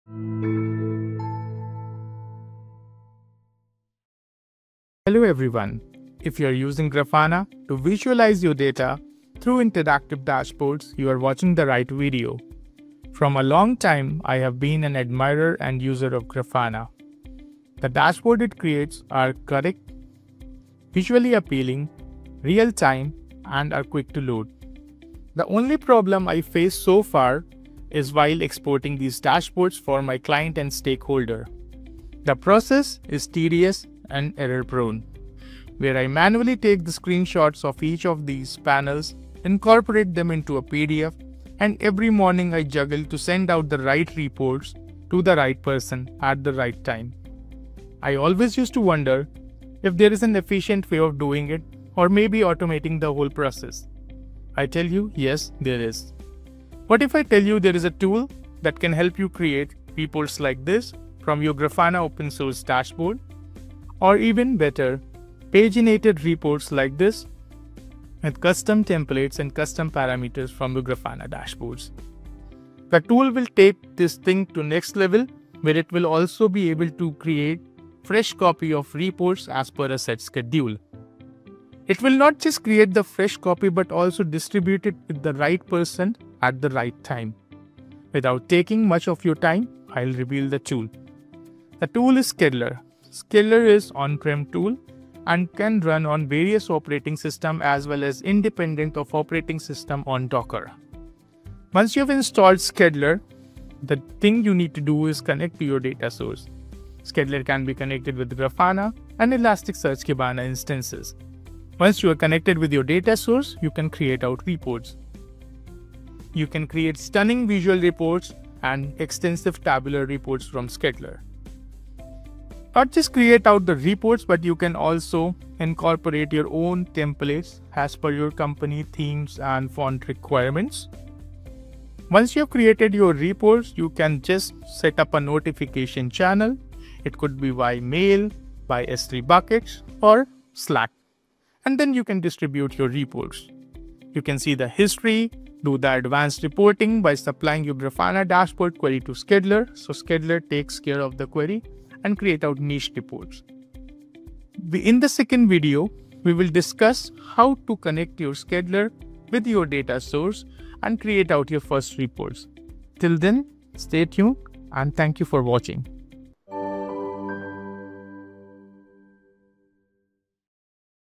Reporting - Export Grafana dashboard | Schedule, Email PDF, EXCEL
Learn how to export your Grafana OSS dashboards as professional reports — in PDF, PNG, and other formats.
In this video, we’ll walk you through a simple and efficient way to automate reporting from Grafana Open Source, without switching to the paid version.

What you'll learn:

How to generate reports from Grafana OSS dashboards

Export options: PDF, PNG, CSV

Automating report delivery via email or chat

Use cases for DevOps, IT teams, and security analysts

✅ Ideal for:
Grafana users | DevOps engineers | SREs | IT admins